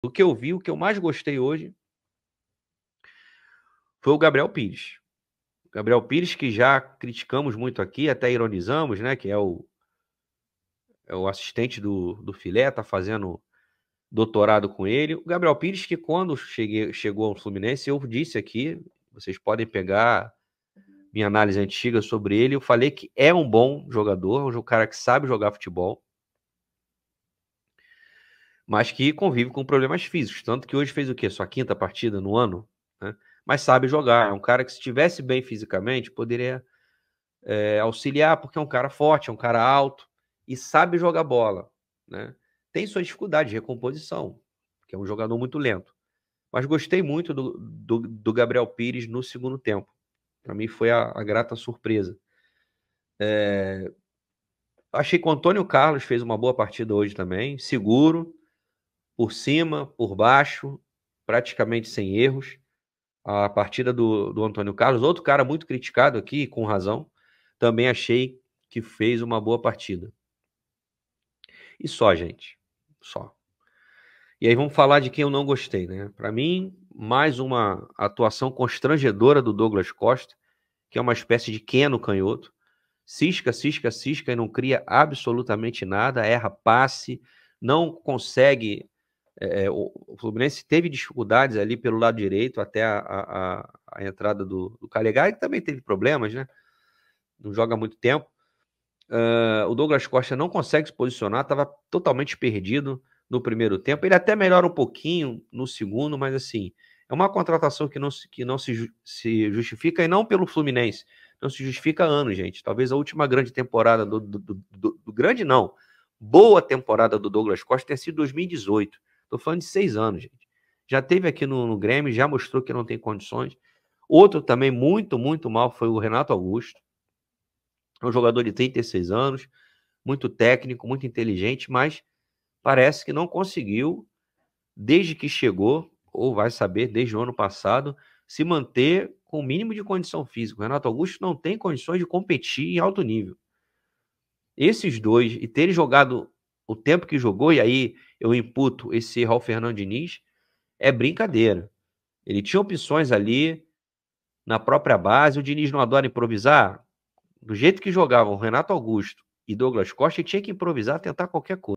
GABRIEL PIRES E ANTÔNIO CARLOS FORAM BEM; RENATO AUGUSTO E DOUGLAS COSTA HORRÍVEIS!
Leandro Dias, editor e comentarista do NETFLU, avalia quem se salvou na derrota para o Cruzeiro por 2 a 0 e quem jogou muito mal. A partida aconteceu em 19/06 pela 10ª rodada do Campeonato Brasileiro de 2024.

Baixe aqui o aplicativo do NETFLU!

Veja notícias em tempo real sobre o Fluminense, calendário dos jogos, vídeos, opinião e podcasts no nosso